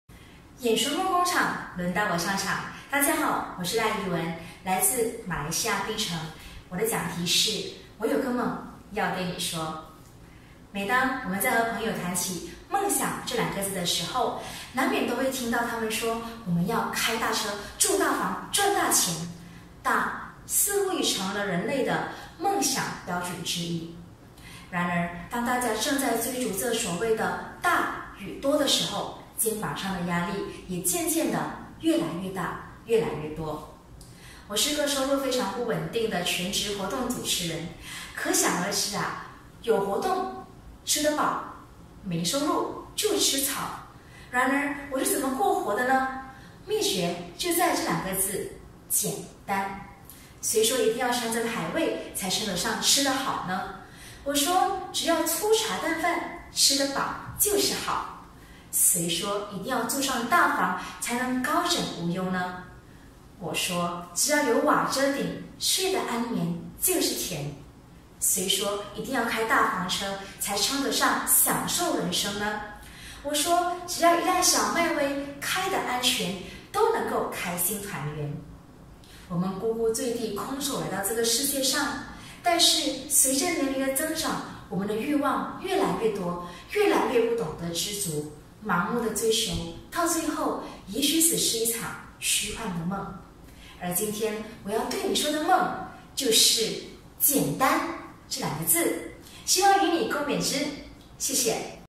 【演说梦功场3】梦想078号， 赖仪雯，我有个梦要对你说。
第3届 演说梦功场
全球大马华人华语视频演说大赛2019